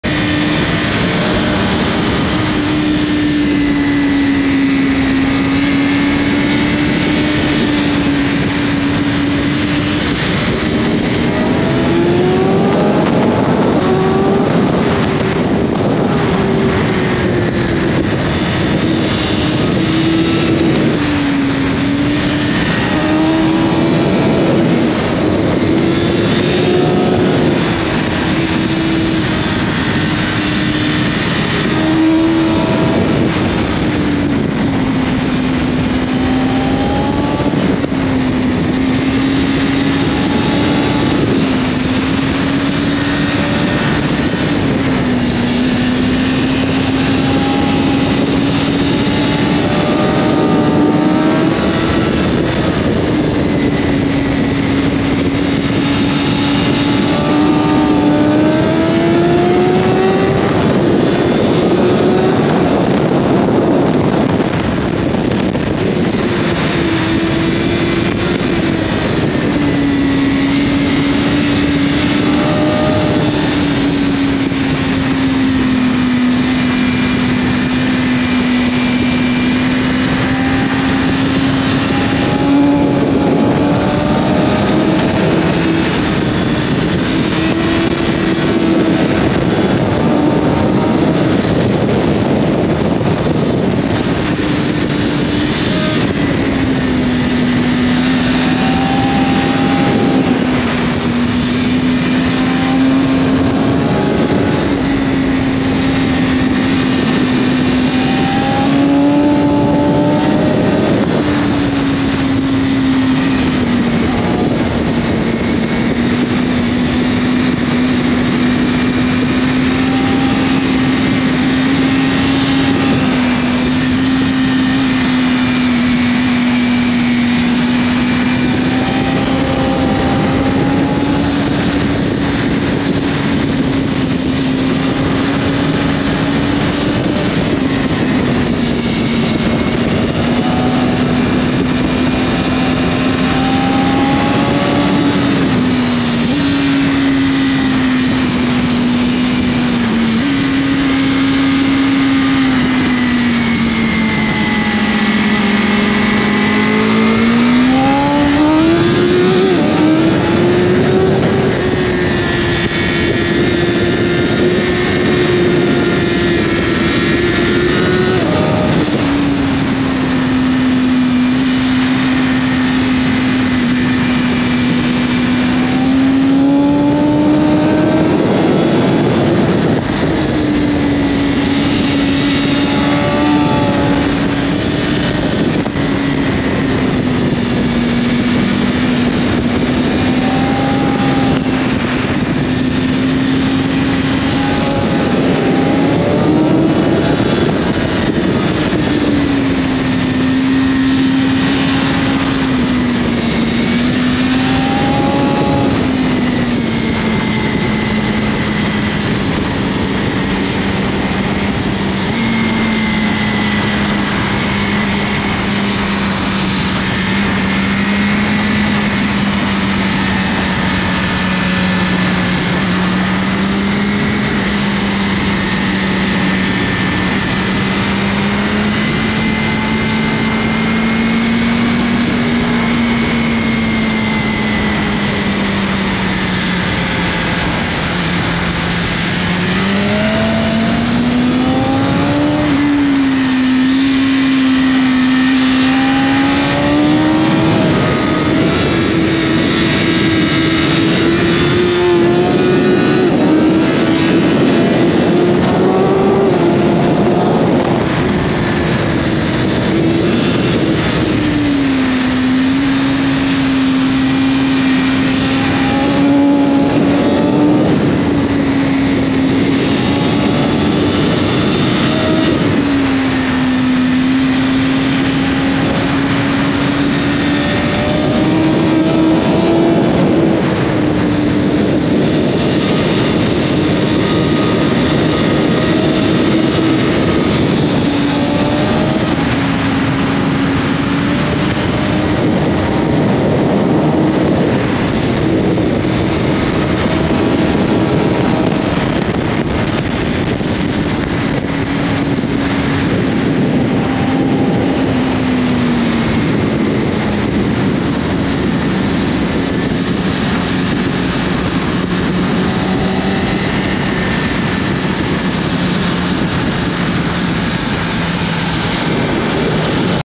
S1000RR vs Streetfighter V4
Chasing Ritchie on his V4 with Supercorsa SC3

BMW on Bridgestone rs11